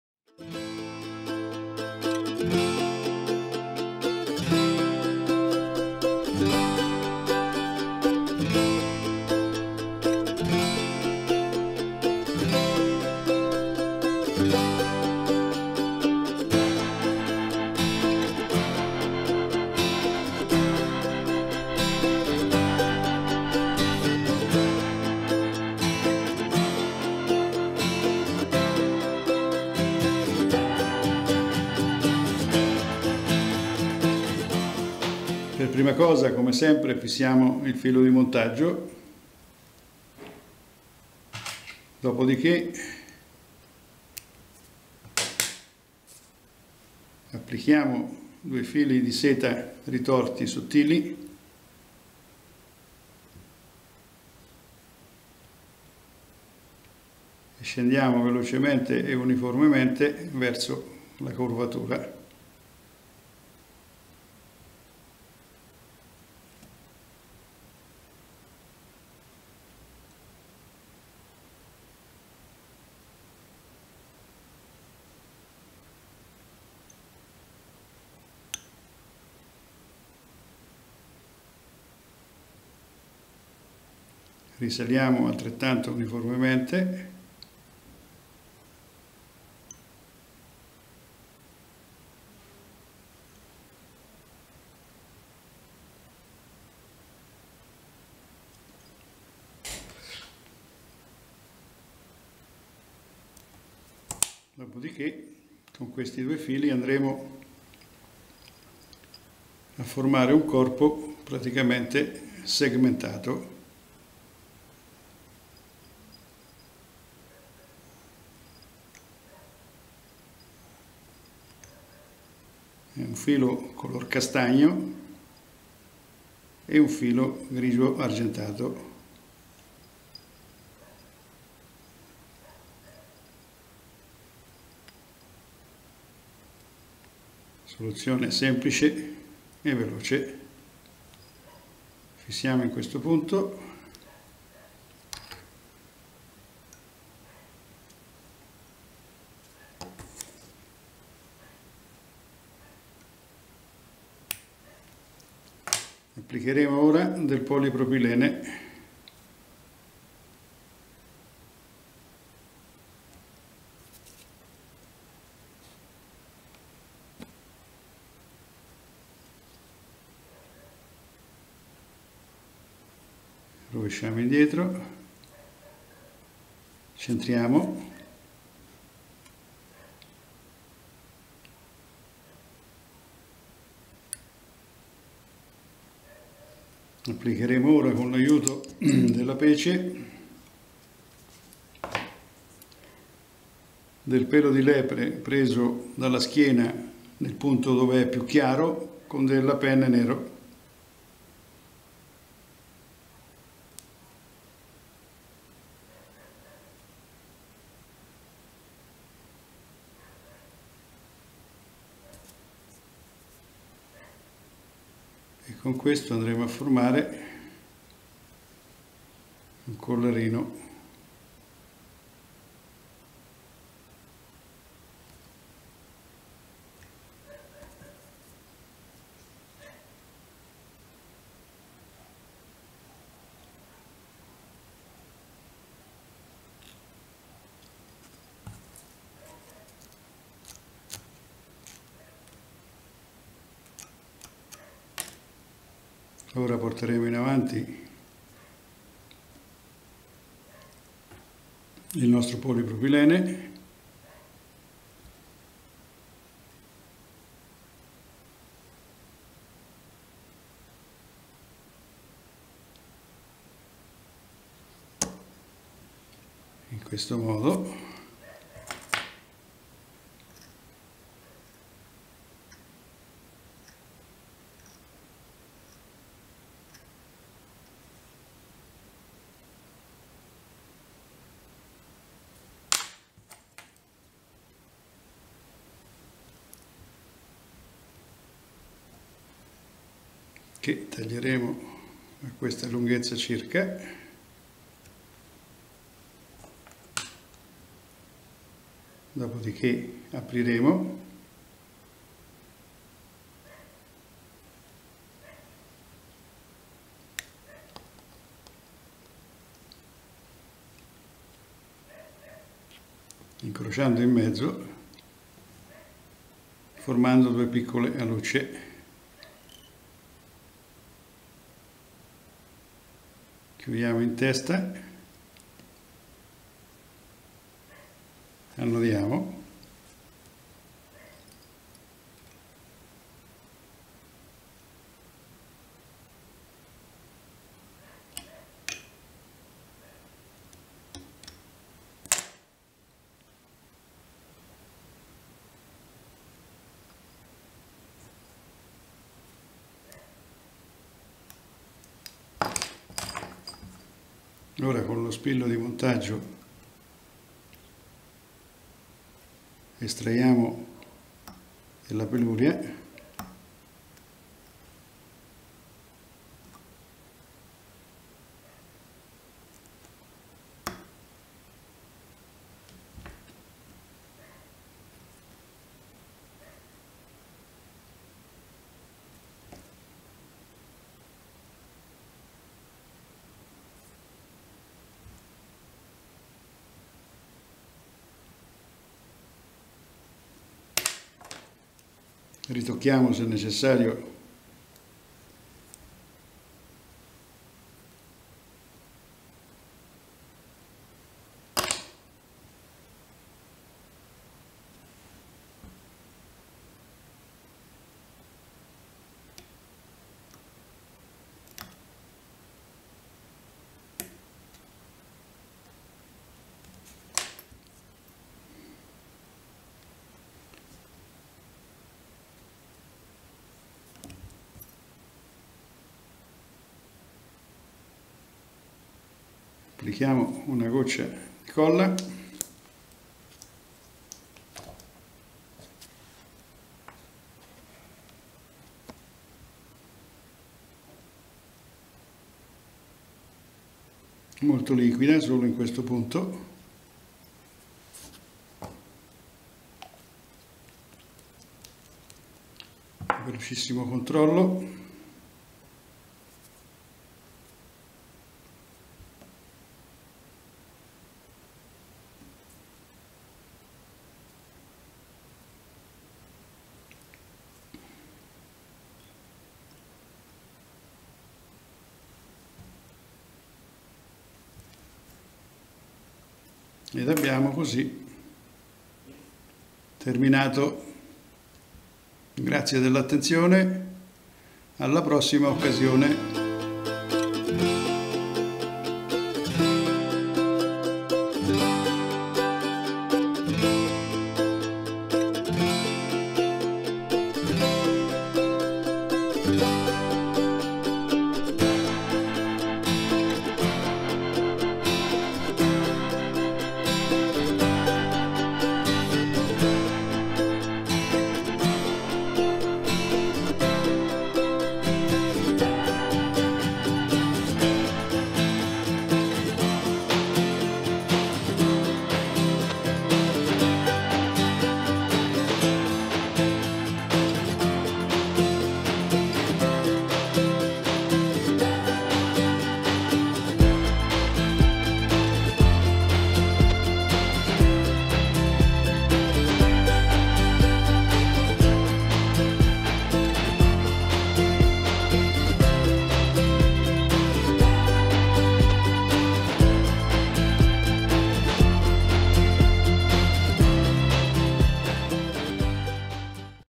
NEWBORN FLY By Giancarlo
Questa imitazione è nata per i laghi di montagna, ma si e rivelata ottima in tutte le situazioni, soprattutto in presenza di pesce estremamente selettivo in acque piatte in generale.

MATERIALE: Ami standard corti 18/16/14/. Addome formato da due fili di seta ritorta, uno avorio e l'altro color senape. Torace pelo di lepre chiaro misto a lapin nero. Sopra torace e ala polipropilene.

MATERIAL: Hooks standard short 18/16/14/. Abdomen formed by two wires of twisted silk, one ivory and the other mustard. Chest, hare rabbit fur mixed with black light. Above chest and wing polypropylene.